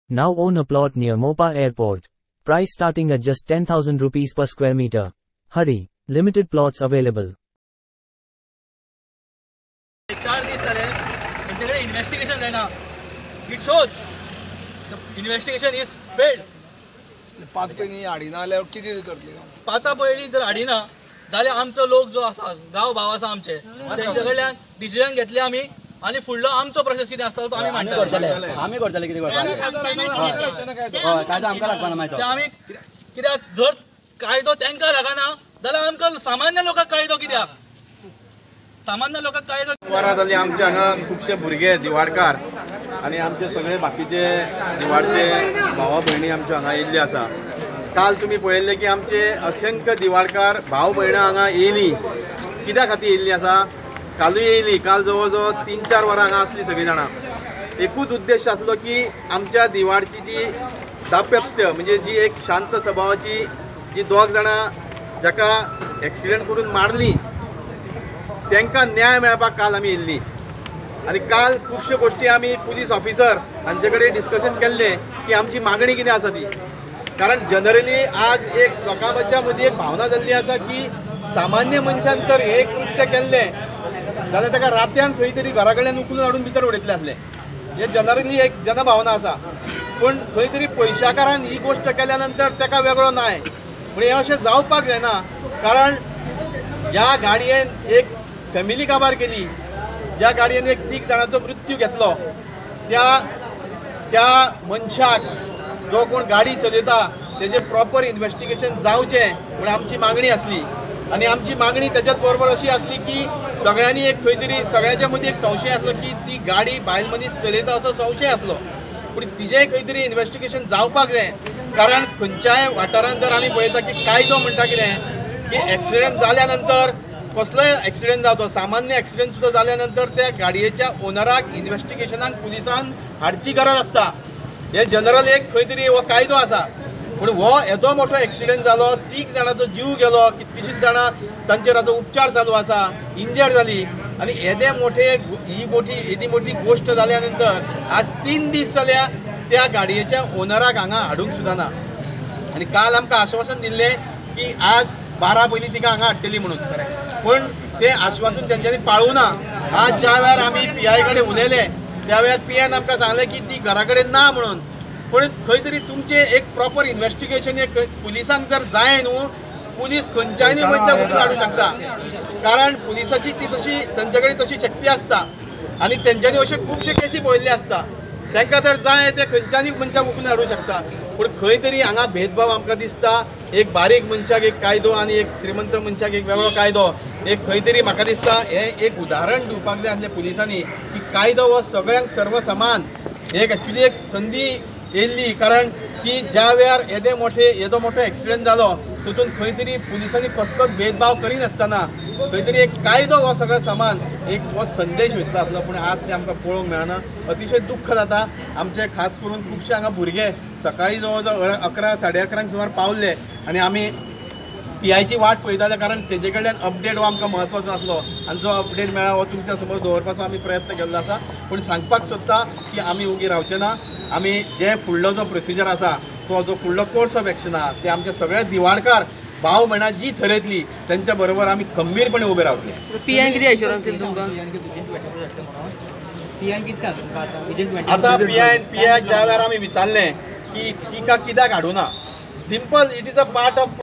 Meghana Sawardekar files for anticipatory bail application!
Meghana Sawardekar files for anticipatory bail application! Police claim that they are still searching for her